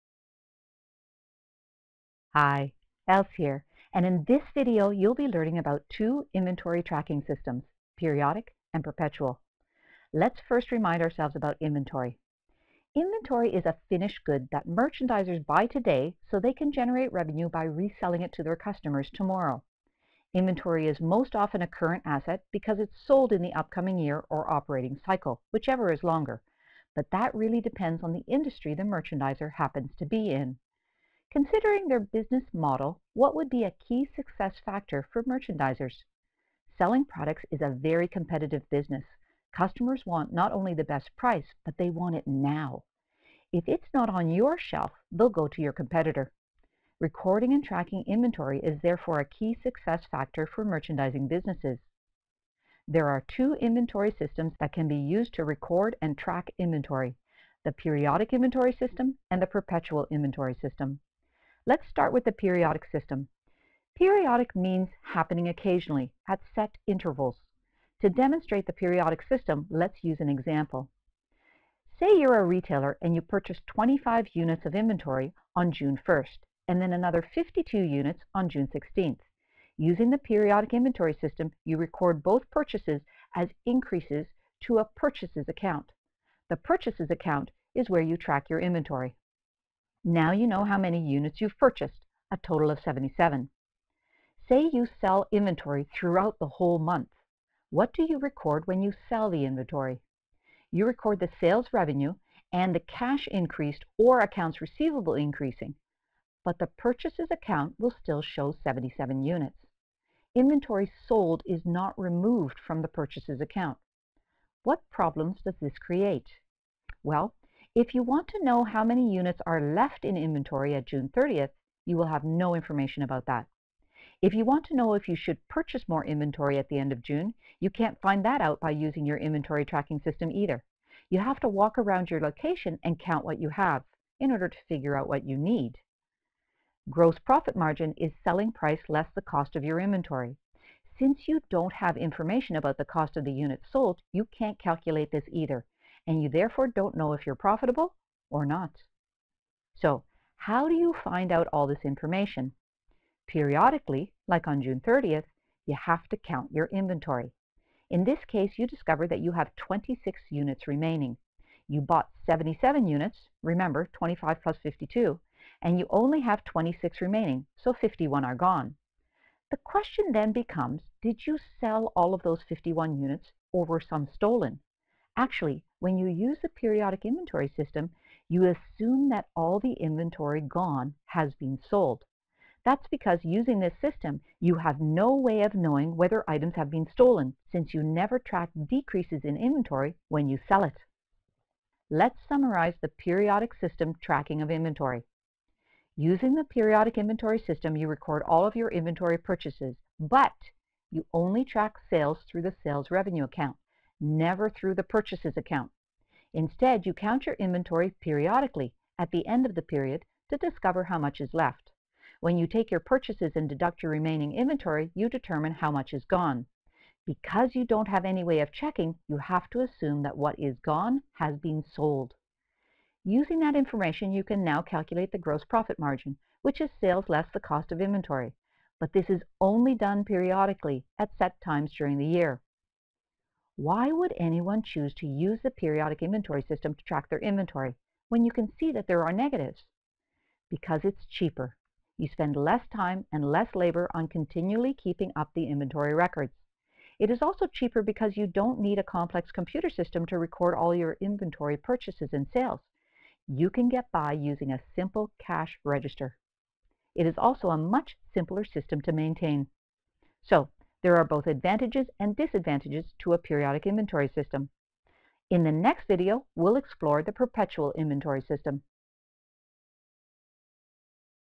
Chapter 5 - The Periodic Inventory System EXPLAINED!
There are two inventory tracking systems for merchandisers.  This video describers the periodic inventory tracking system using an example so that you can clearly see how it works, what the benefits are but also the drawbacks.

This video was produced for ACC100, introductory financial accounting, but can also be used by other students who need to better understand the two inventory tracking systems, their benefits and drawbacks.